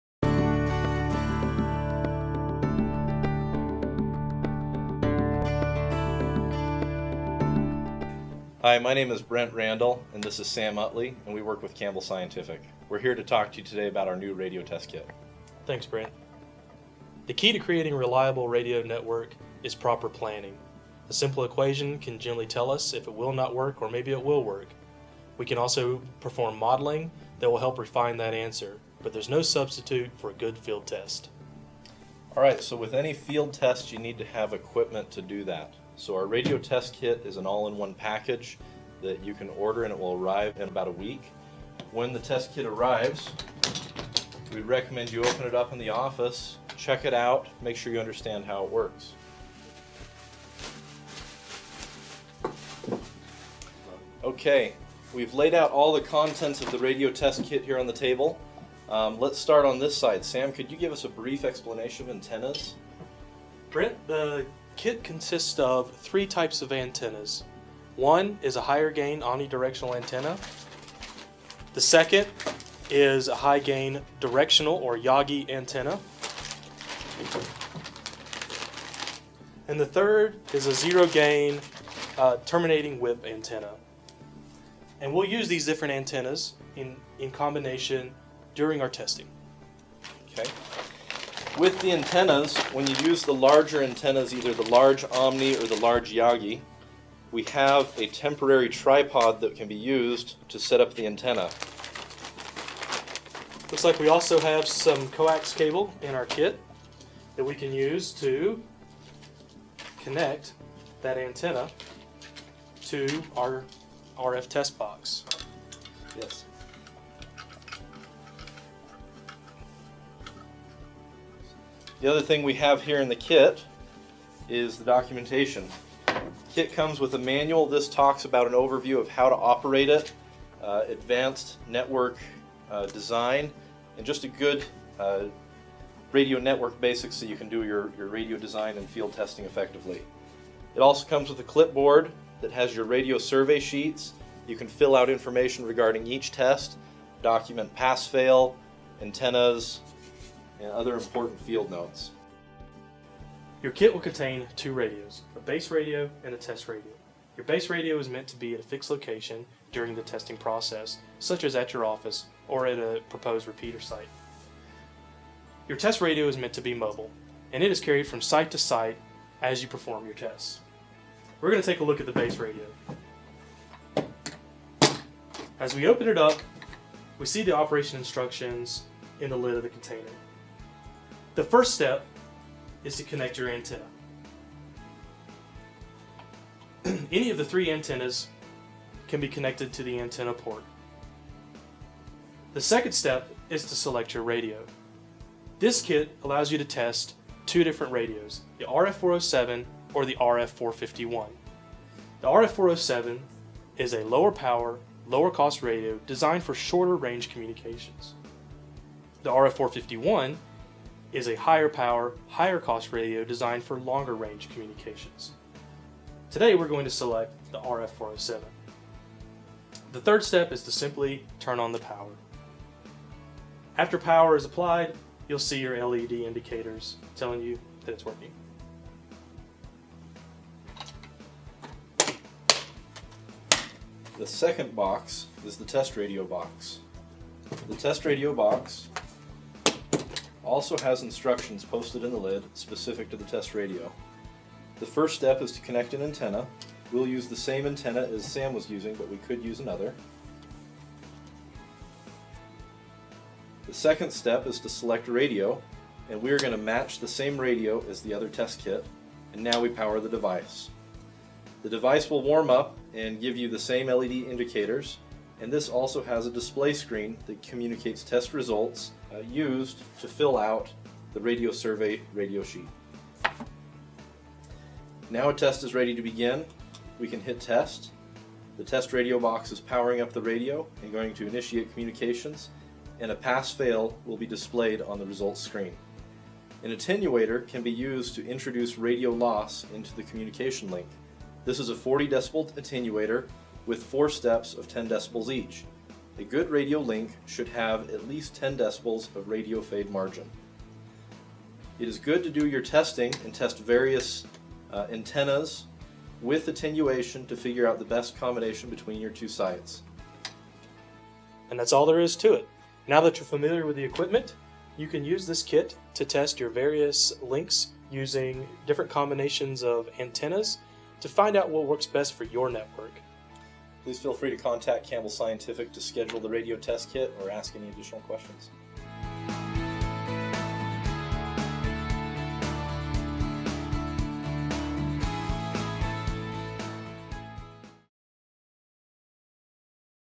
Using the Radio Test Kit
The key to creating a reliable radio network for your data acquisition system is planning. Campbell Scientific's radio test kit is the ideal tool for field testing your proposed RF407 or RF451 network with various antenna combinations.